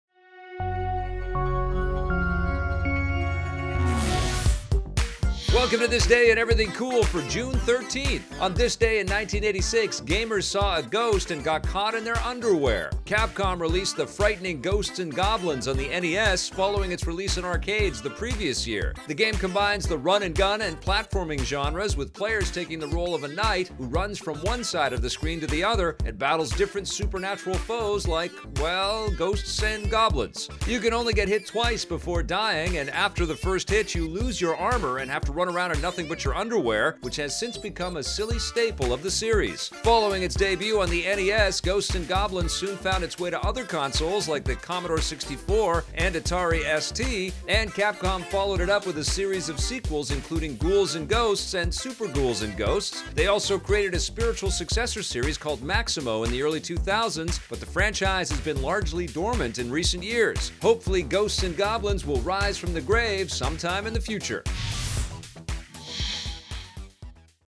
Ghosts, Goblins, and Ghouls! - This Day In Everything Cool for June 13 - Electric Playground
Capcom unleashes ghosts, goblins, and ghouls! In this video, we find out the coolest thing that happened on June 13!